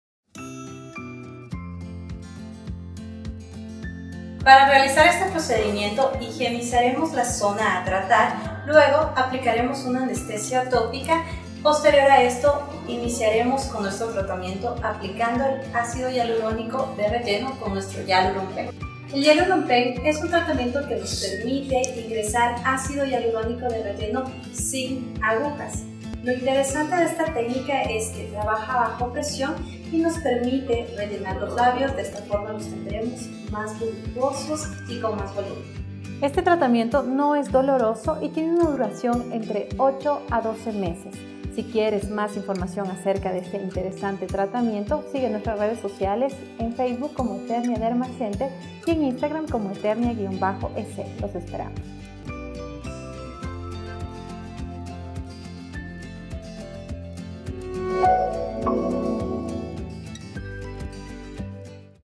¿Cómo aumentar tus labios con Hyaluron Pen? - Tratamiento de relleno de labios en Quito
Aumento de Labios con HYALURON PEN.  El Hyaluron Pen es una solución óptima para personas con piel sensible que se inclina a tener cicatrices y hematomas. Sin agujas y sin dolor.